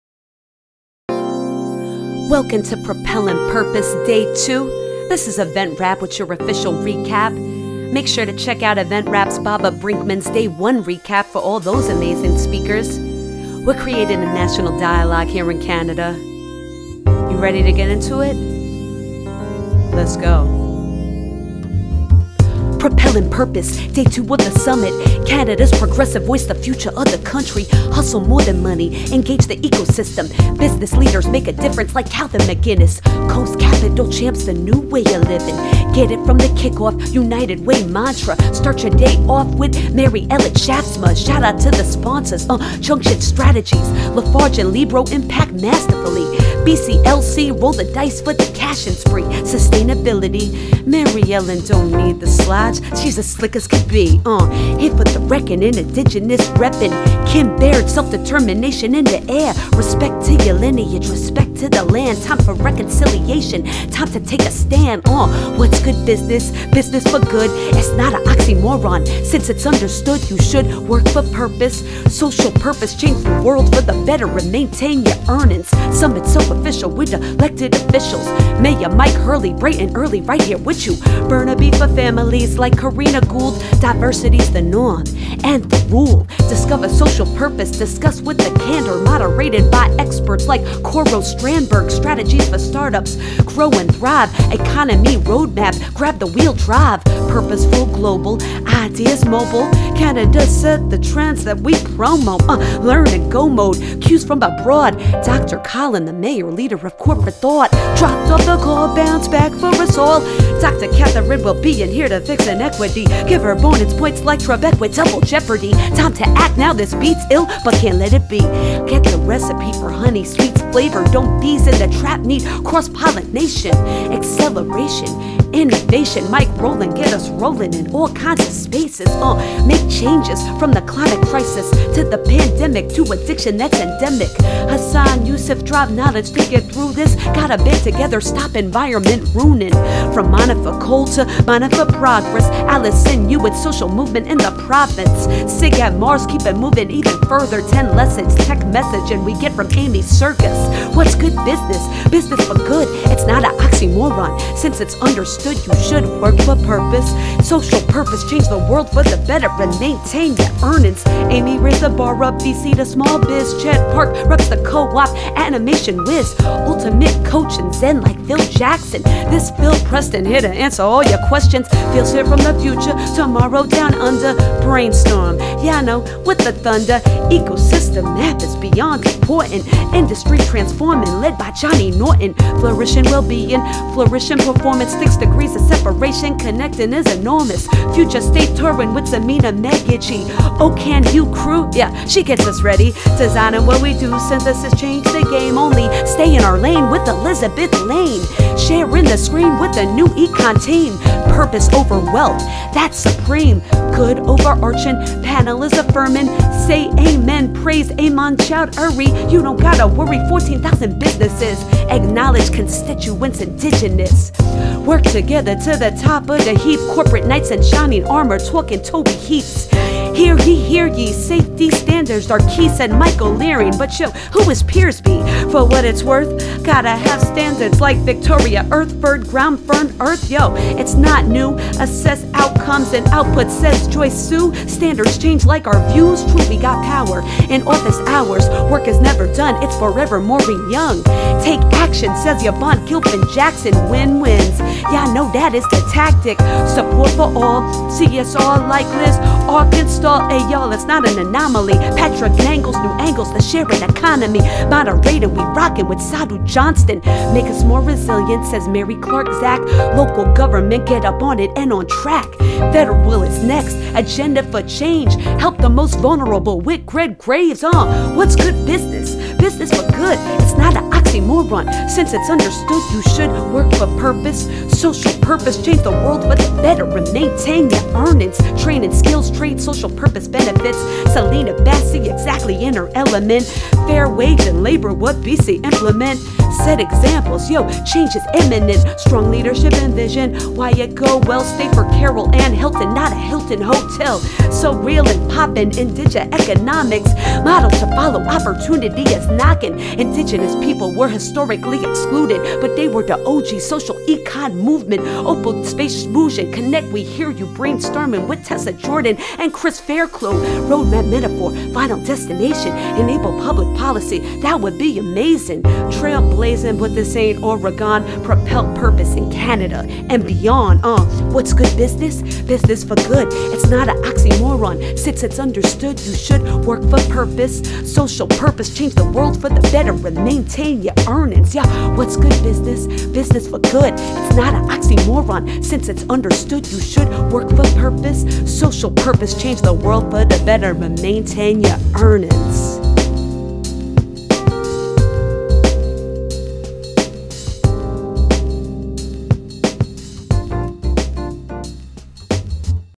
Propelling Purpose Summit 2021 Rap Up Showcase - Day 2
Lex Rush raps a live summary of the talks at the United Way Social Purpose Institute's first-ever Propelling Purpose Summit, Day 2, where businesses change their targets and activities to align with a Social Purpose.